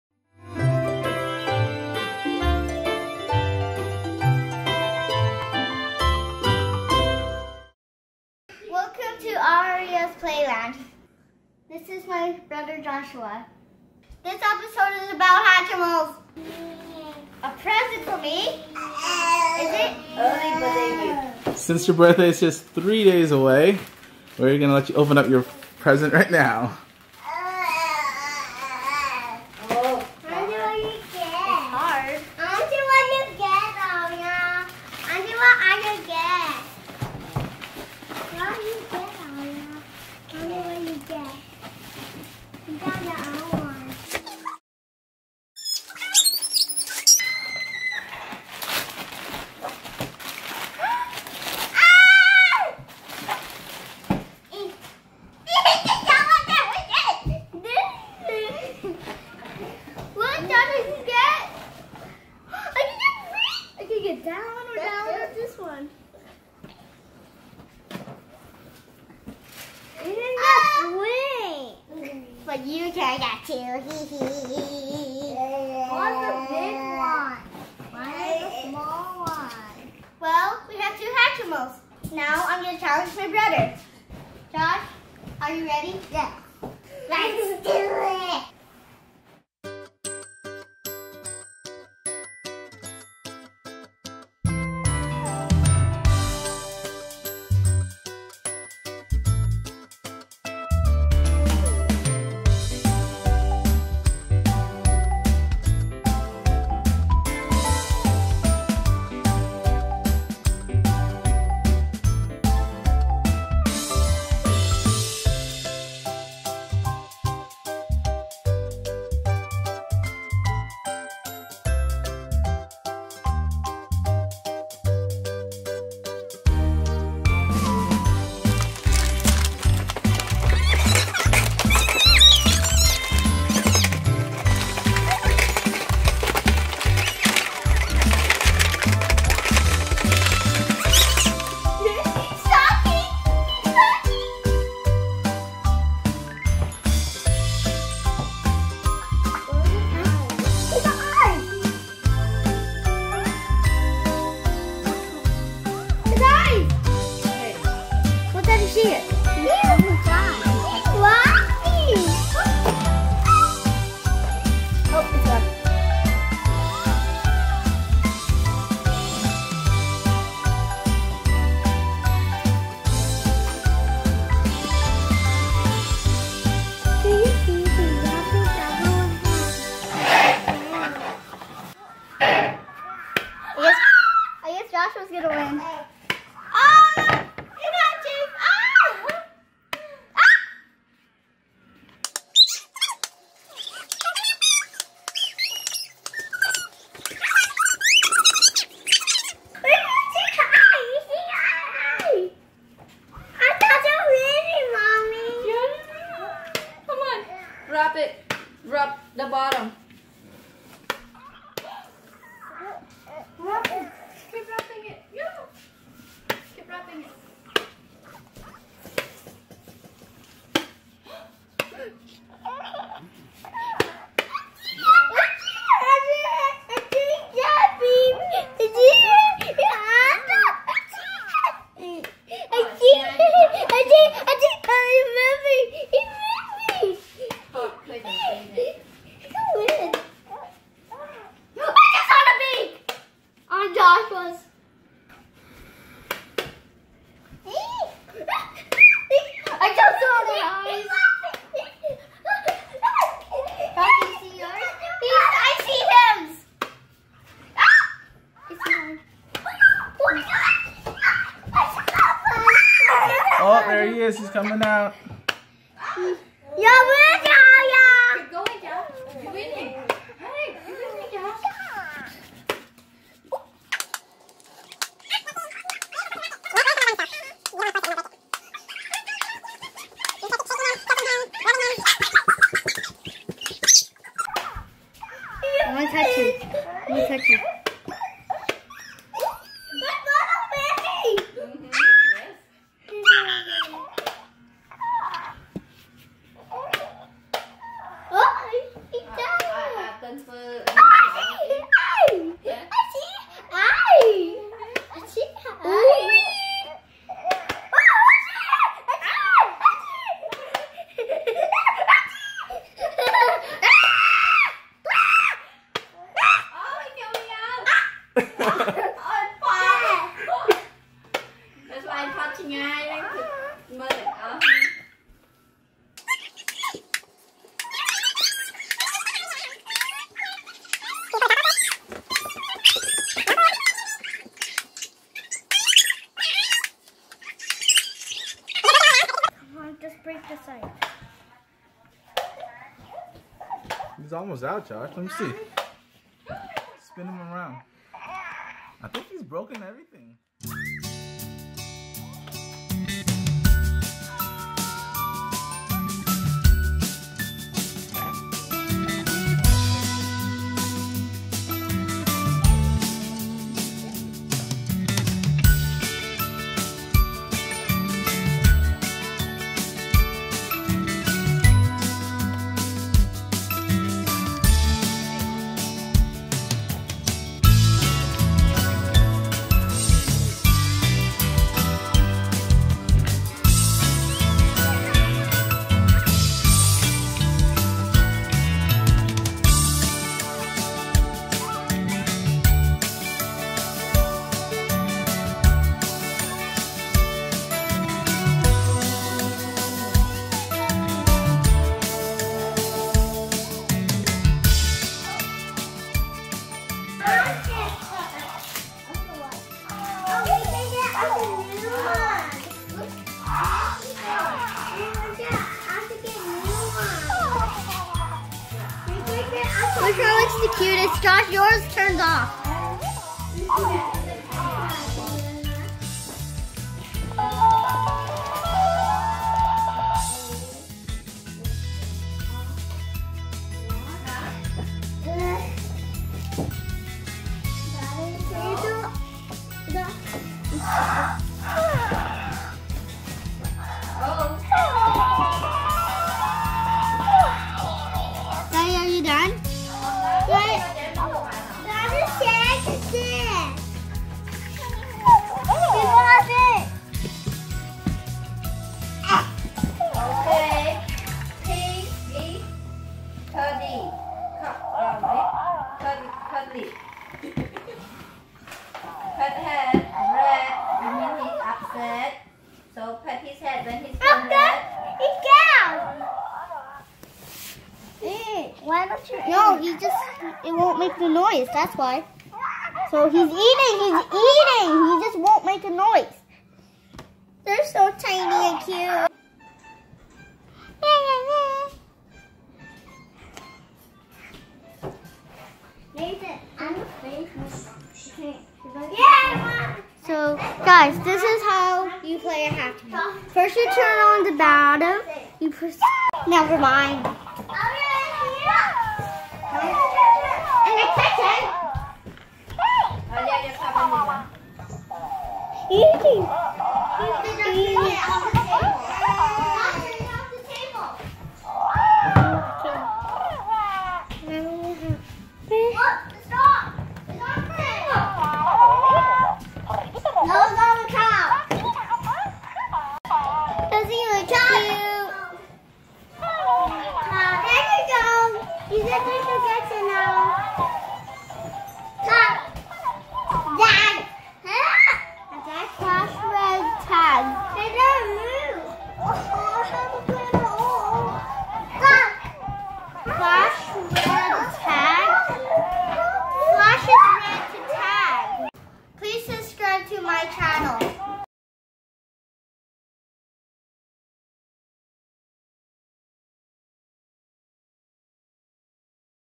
Hatchimals
Aria gets a surprise gift as we prepare to Hatch our first Hatchimal. Then the race is on to see who can hatch their egg first!